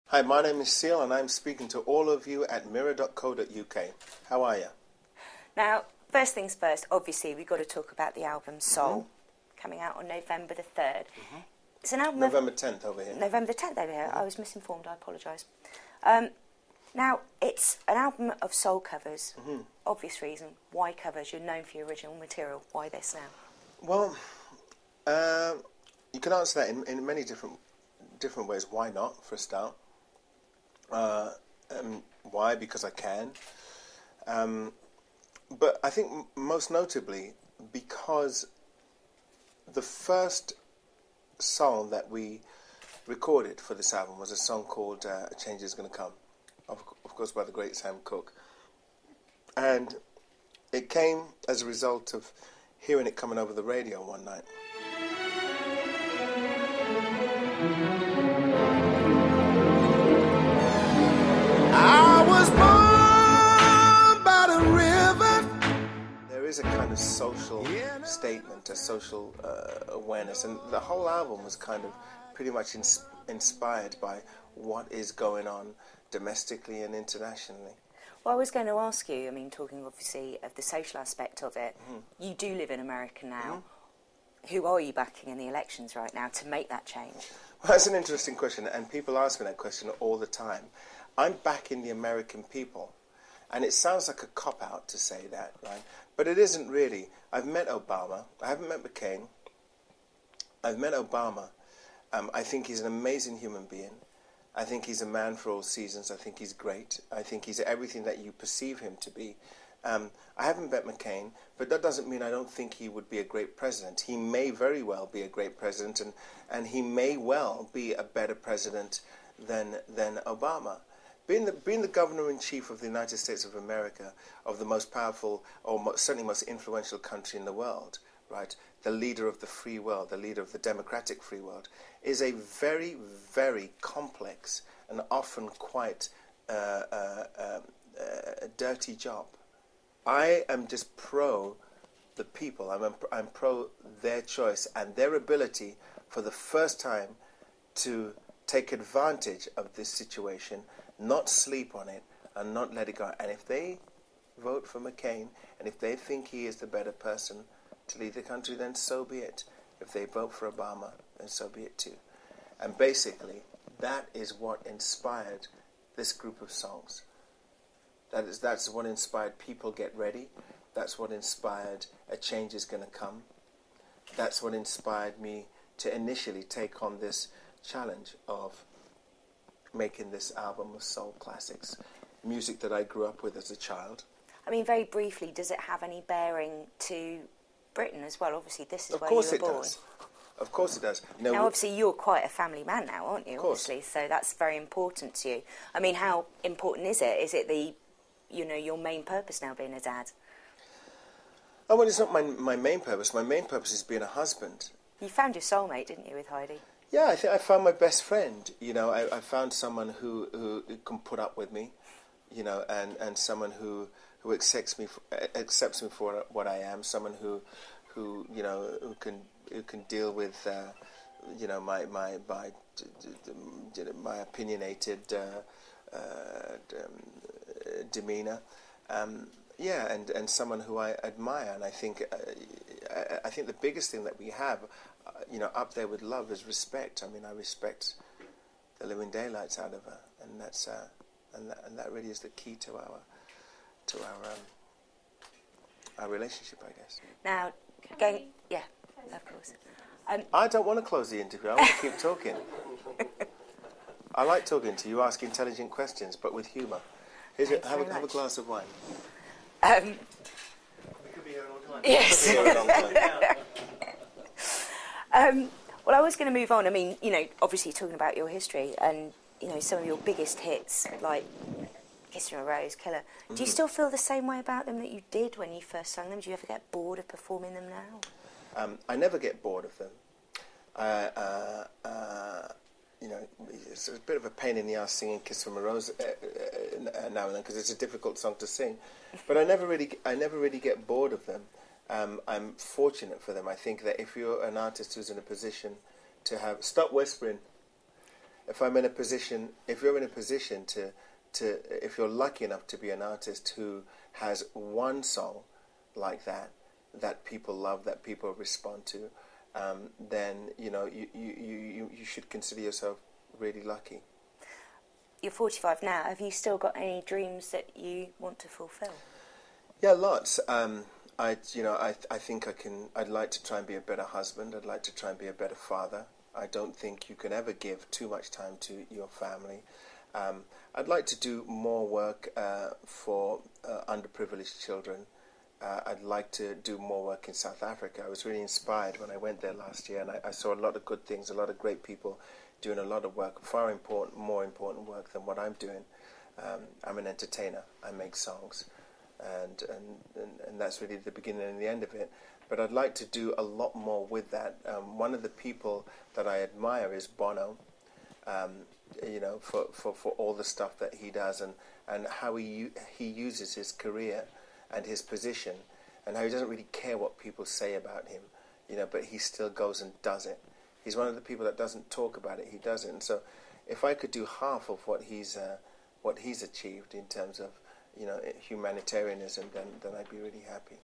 Seal interview
The singer on his love for Heidi Klum, the US poll and being the next Bono